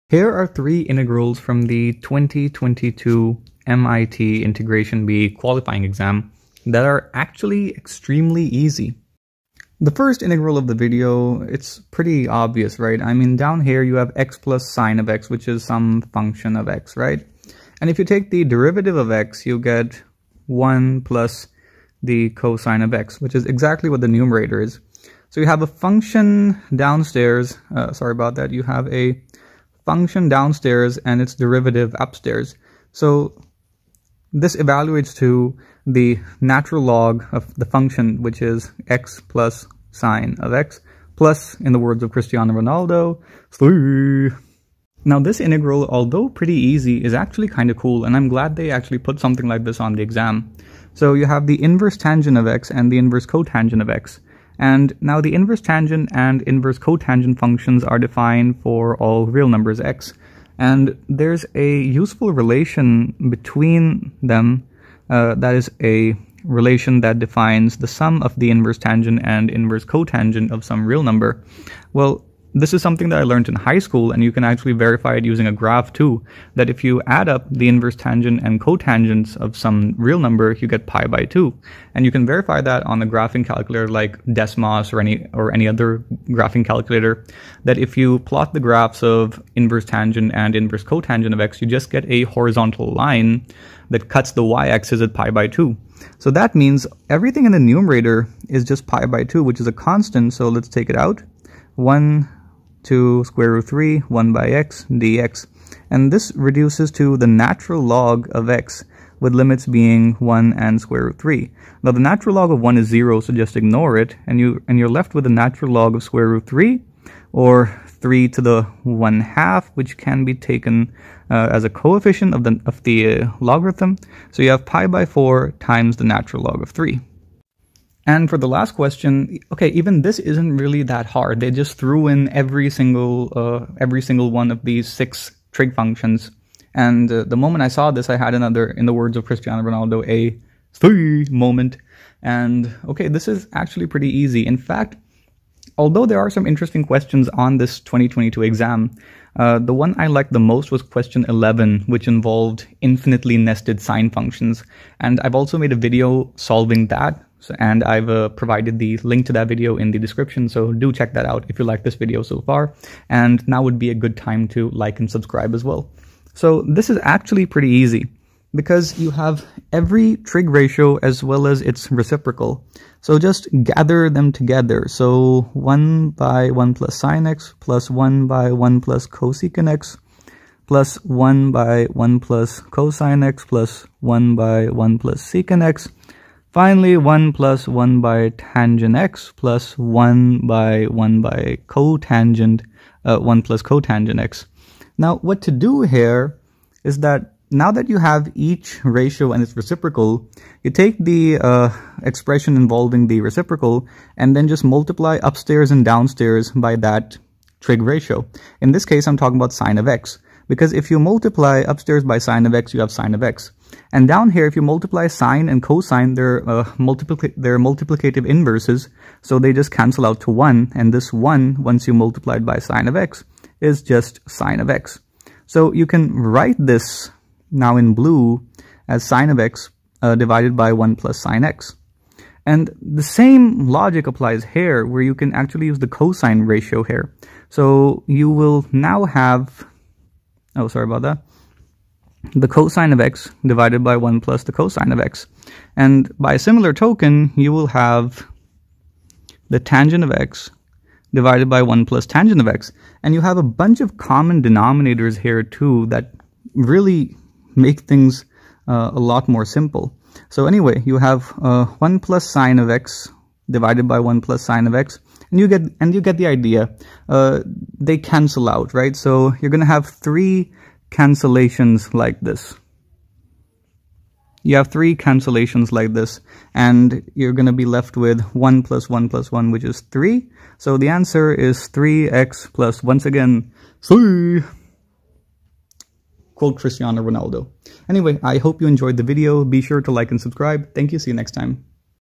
Is the 2022 MIT integration bee qualifying exam too easy?
This video is about 3 trig integrals from the 2022 MIT integration bee qualifying exam that are actually pretty easy to solve using basic math and calculus knowledge.